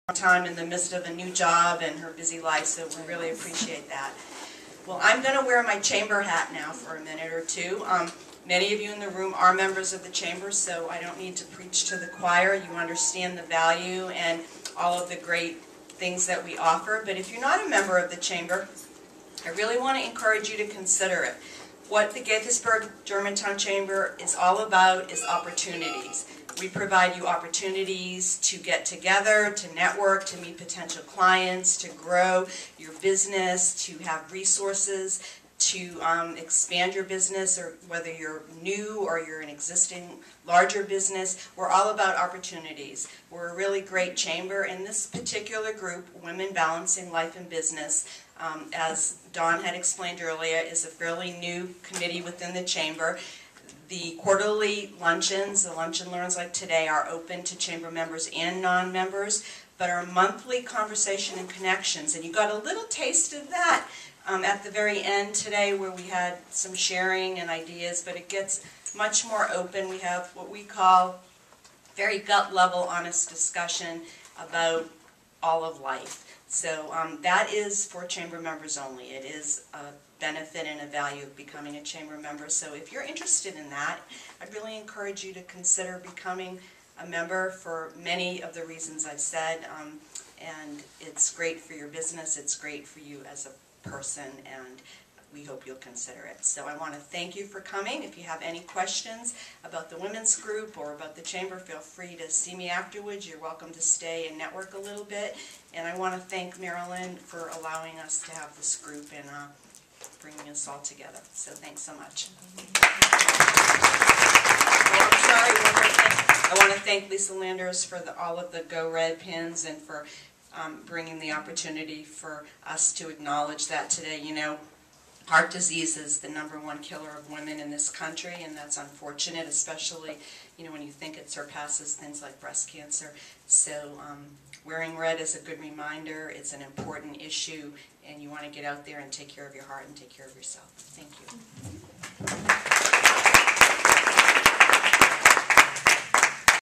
GGCC Director of Member Services talks about the Chamber's new Women's Group
The Women's Lunch and Learn is a quarterly activity of the Gaithersburg-Germantown Chamber's committee, "Women Balancing Life and Business".  Our luncheon is open to members and non-members and will be held at the Wyndham Garden Hotel, 805 Russell Avenue, Gaithersburg. For more information or to attend this luncheon, please visit the GGCC Price for admission is $30.